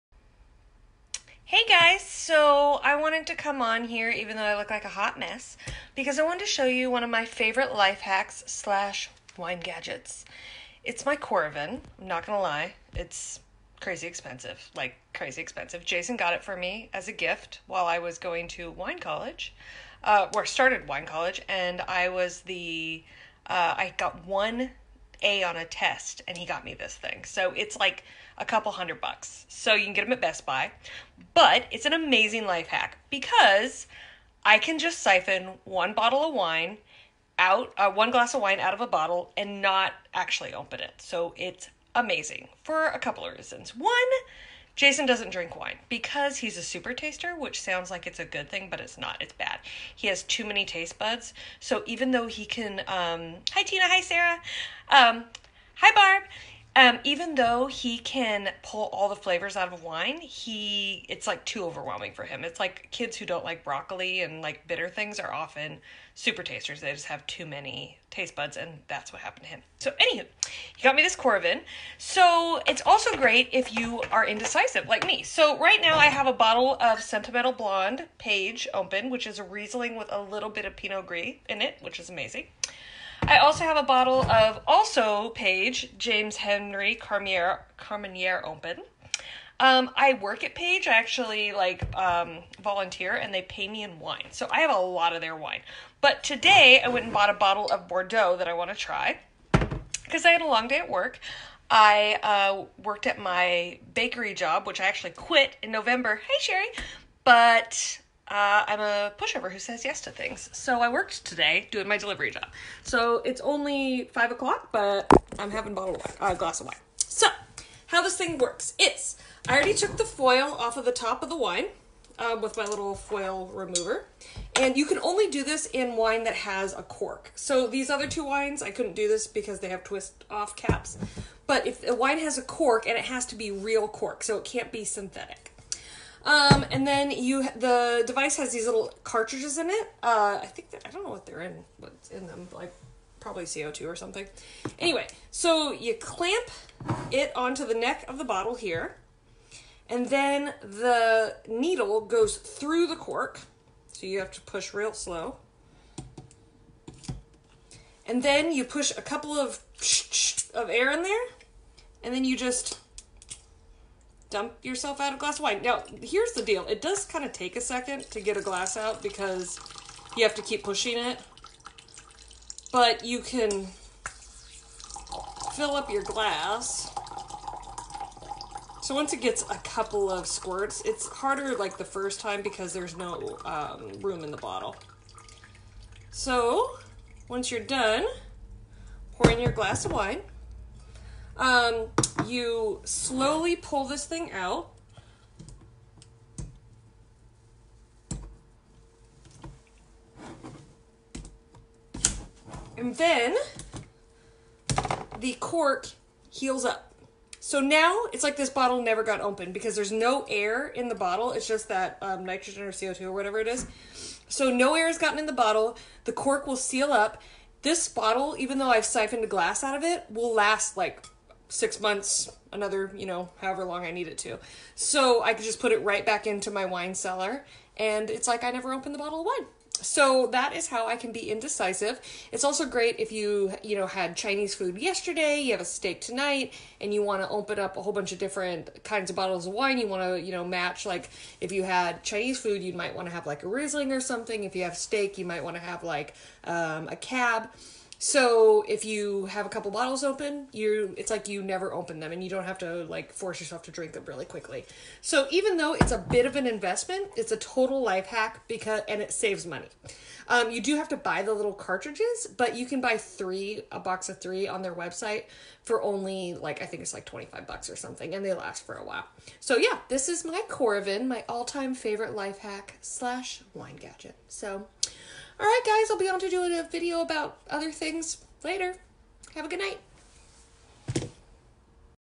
My Coravin!! My favorite wine gadget
How to use a Coravin and why it's my favorite wine gadget